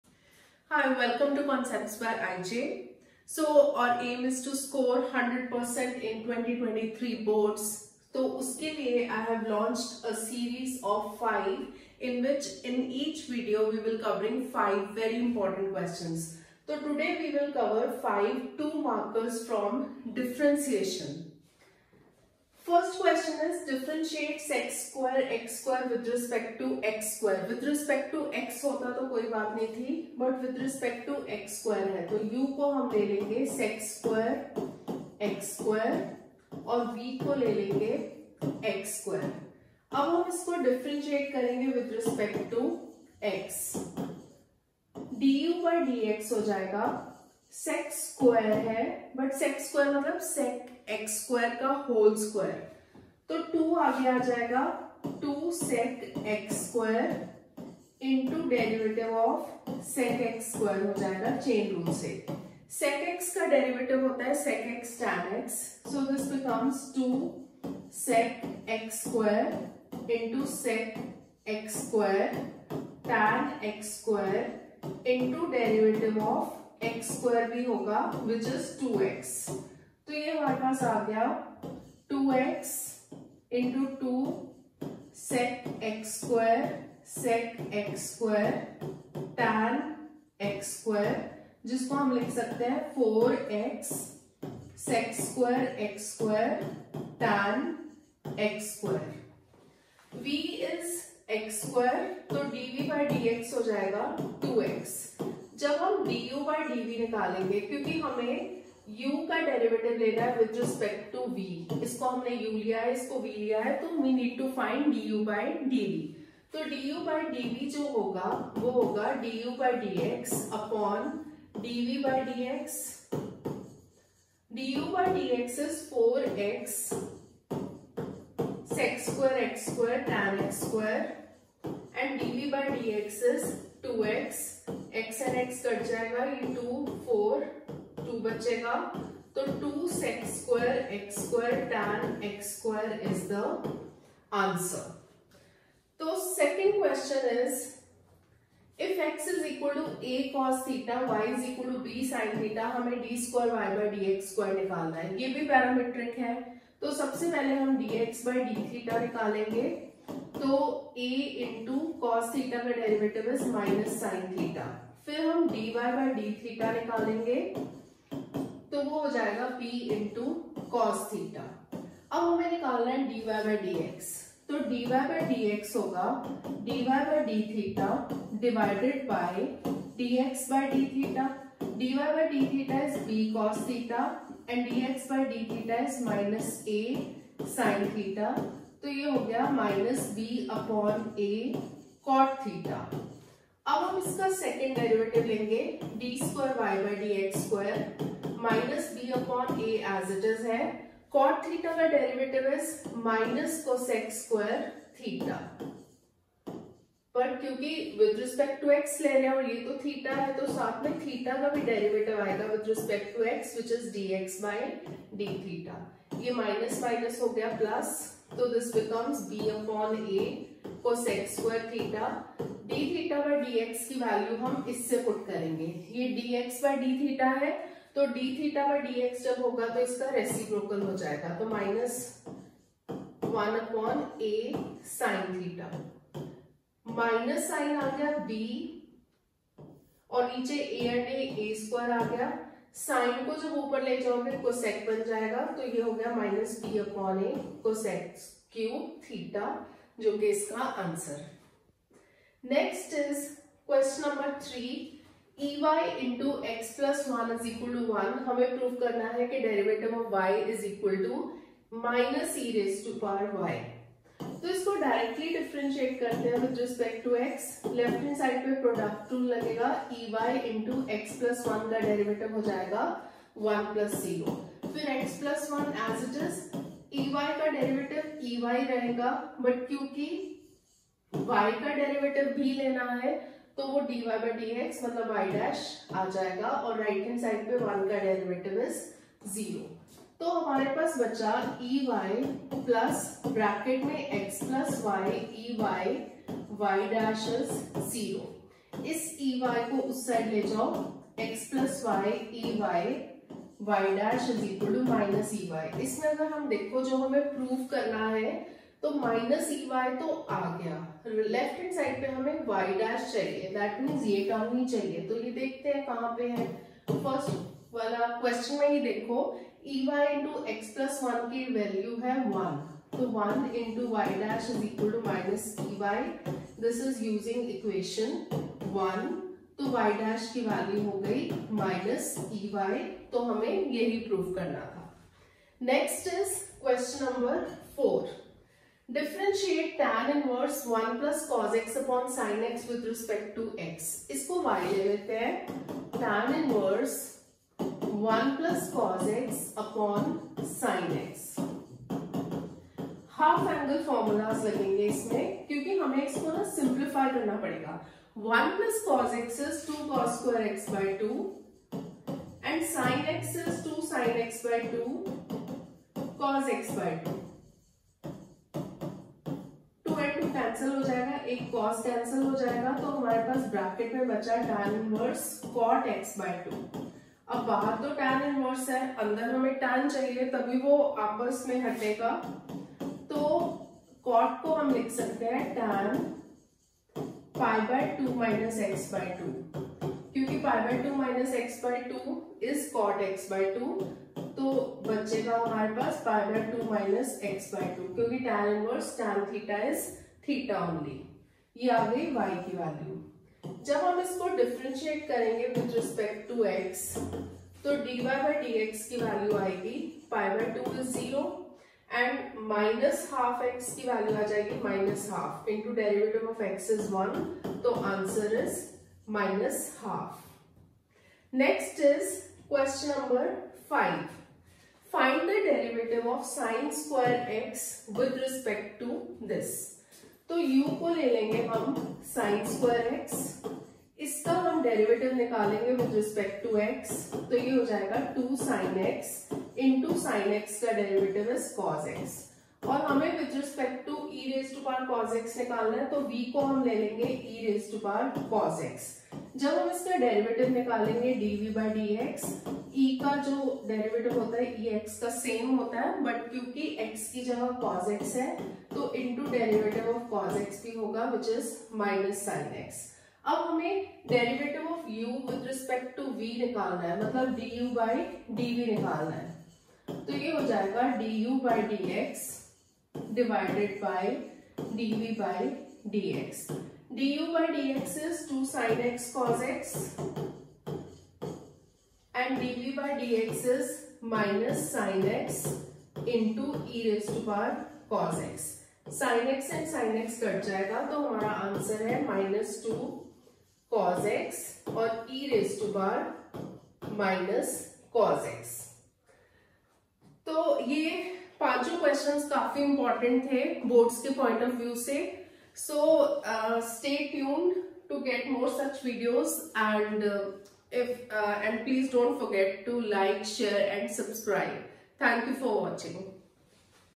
DIFFERENTIATION | TWO MARKERS| CLASS 12 CBSE | MATHS BOARDS 2023 | CUET 2023
To ensure complete revision for 2023 Maths board exam and 2023 CUET exam, a video series named SERIES OF 5 has been started. In each video, very important 5 questions of a particular topic will be covered.
In this video, 5 important differentiation questions have been covered which are very important from boards point of view. These are all two markers which are very important and can be asked in boards.
Hi, I am Indervir Jolly, an M.Sc. in Mathematics. I have been teaching Maths for the last 23 years having a full grip on the trends in Board exams.